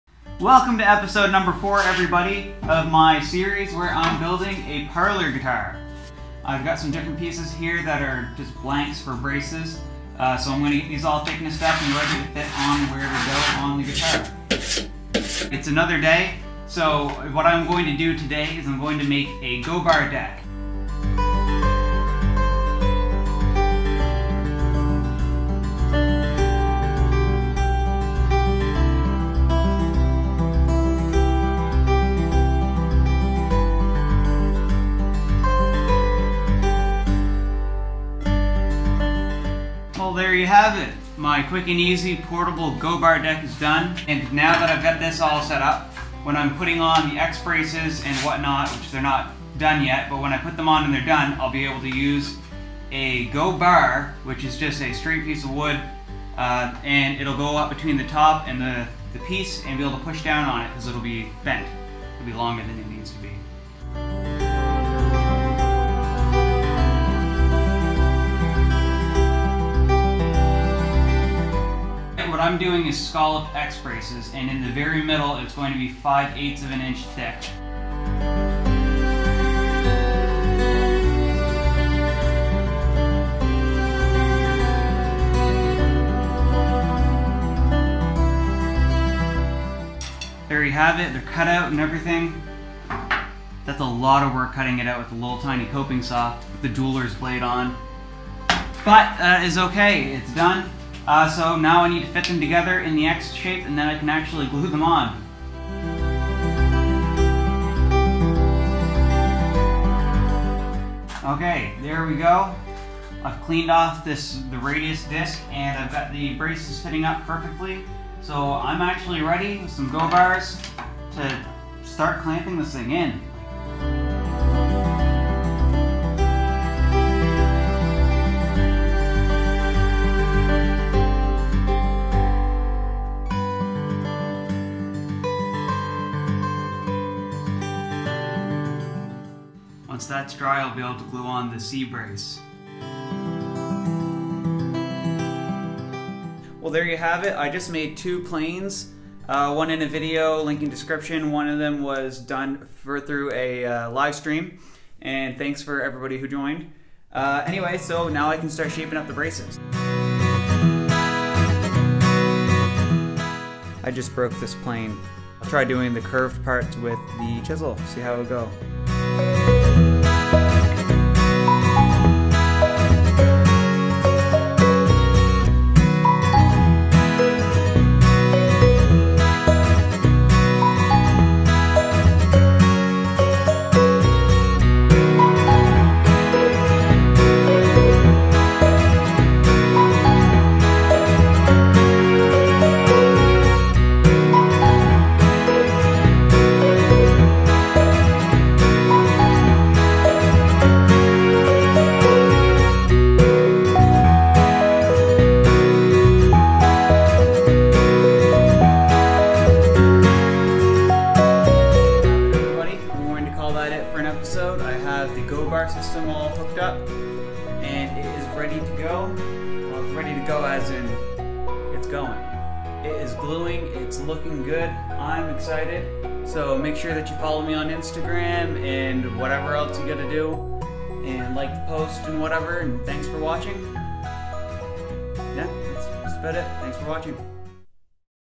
Acoustic Guitar Build | Part 4 | Nova Scotia Parlour Guitar
If you have ever wondered how to make an acoustic guitar, what it takes to build a guitar, then watch this video. In this video, Daniel installs the braces for the Nova Scotia Parlour Guitar, this is an acoustic guitar that he is building with all Nova Scotia wood. Please enjoy watching this adventure back into building guitars!

The content of this video is not intended to be followed as an educational guide, it is merely entertainment.